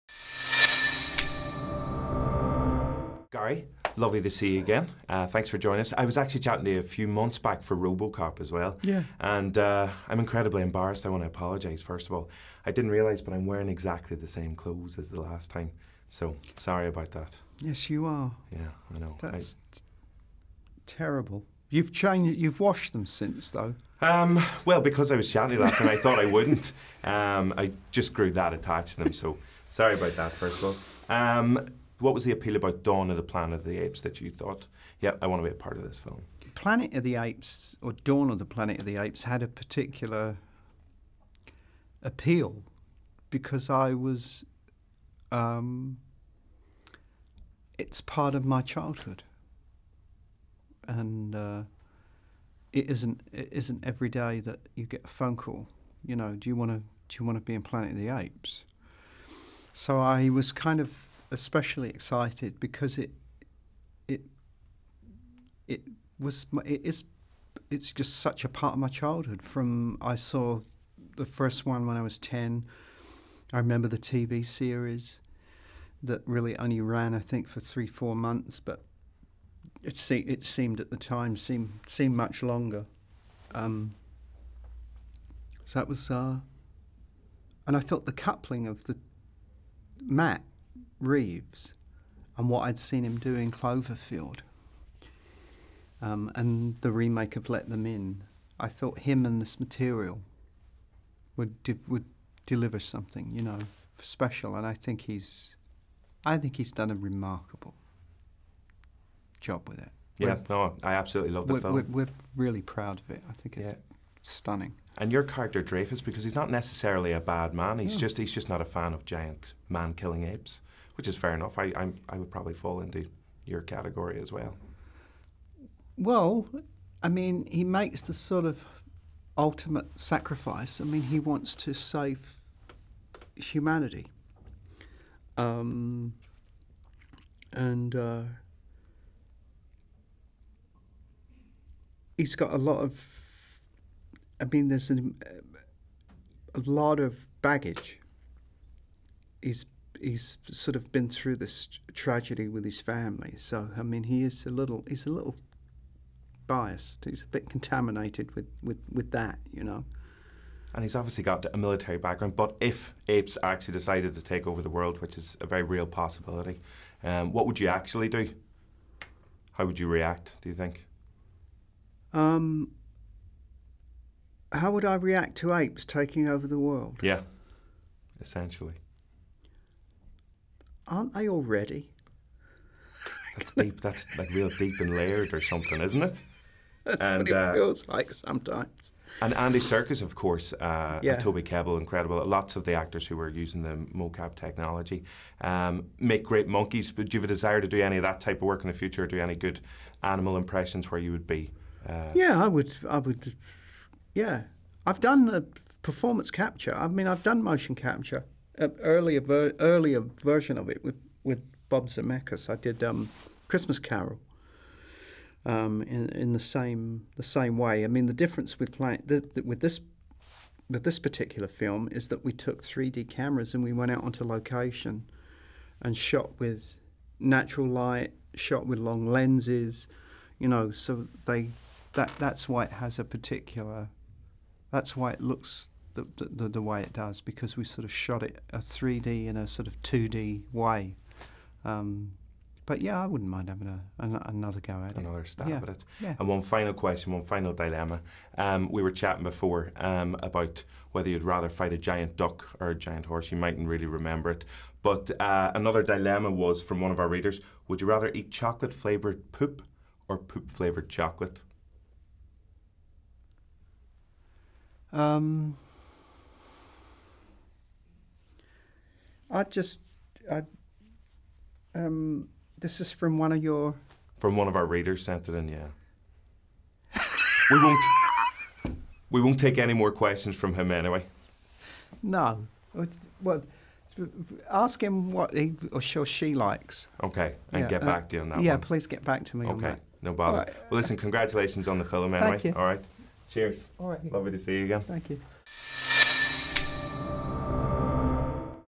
JOE meets Gary Oldman, star of Dawn Of The Planet Of The Apes
Seriously, we're not monkeying around.

The superb Dawn Of The Planet Of The Apes swings its way into cinemas this week and, ahead of its eagerly-anticipated release, Eoghan Doherty met up with old pal Gary 'motormouth' Oldman and Matt 'look at my amazing dickie bow' Reeves to discuss all things Apes-related, including the unfortunate demise of the Irish nation as a result of Simian Flu, the dilemmas of eating poop-flavoured chocolate and, most importantly, what are the best monkey-inspired film puns.

It really is hard to beat Closed Encounters Of The Furred Kind if you ask us.

Dawn Of The Planet Of The Apes picks up the awesome ape action ten years after the end of the excellent Rise Of The Planet Of The Apes (2011), joining genetically-evolved Caesar and his colony of loyal furry followers in their new home in the forests outside San Francisco.

Their new-found freedom, however, is soon under threat when a band of Simian Flu-surviving humans stumble across the secret monkey community. The two species reach a fragile peace, but it proves short-lived, as both sides are brought to the brink of Gorilla warfare which will determine who will emerge as Earth's dominant species.

GIRAFFES!

Sorry, no. Not giraffes. It'd be apes or humans.

Speaking of humans, Dawn Of The Planet Of The Apes stars the aforementioned Gary Oldman, Andy Serkis, Jason Clarke, Keri Russell and the incredible Toby Kebbell (incredibebbell?) and we can assure you, this sequel is most definitely one of the must-see films of the year.